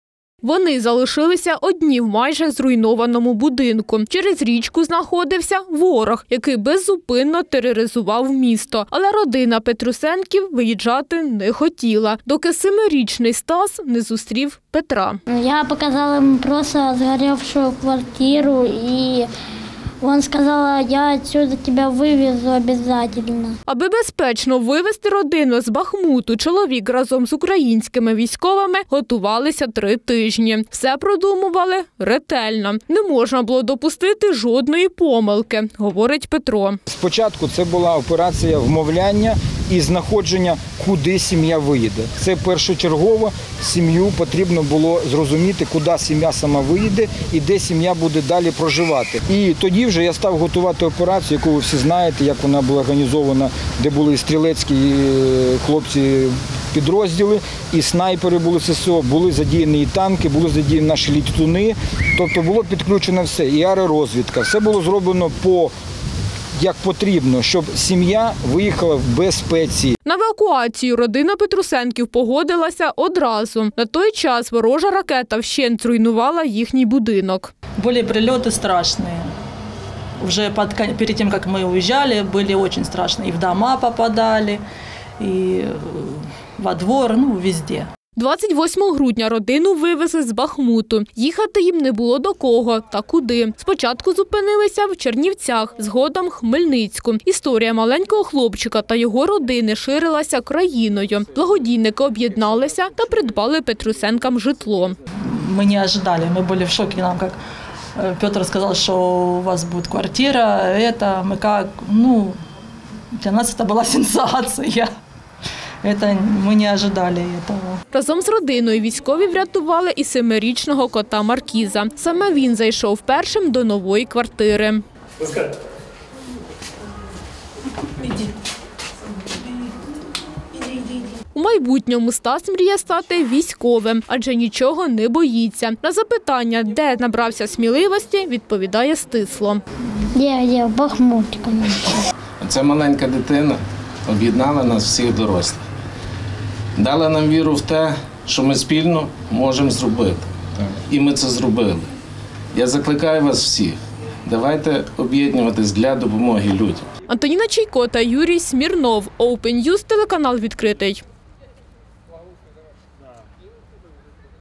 Врятована родина із Бахмуту оселилася в новій квартирі у Дніпрі
Обживаються в новій оселі. Після півроку митарств по чужих кутках родина Петрусенків з Бахмуту отримала квартиру у Дніпрі. Її придбали благодійники. Сьогодні нові власники отримали ключі від оселі та запустили на щастя в помешкання першим свого кота Маркіза, якого не залишили під обстрілами.